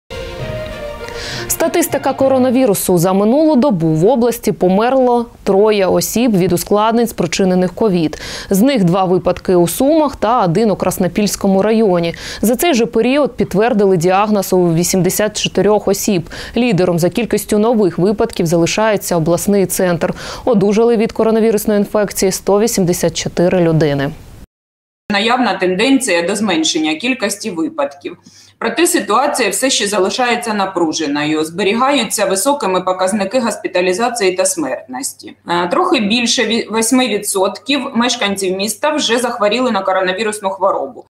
3 людини померли, 84 захворіли: статистика коронавірусу на Сумщині
Статистика коронавірусу. За минулу добу в області померли троє осіб від ускладнень, спричинених COVID. З них 2 випадки у Сумах та 1 у Краснопільському районі. За цей же період підтвердили діагноз у 84 осіб. Лідером за кількістю нових випадків залишається обласний центр. Одужали від коронавірусної інфекції 184 людини.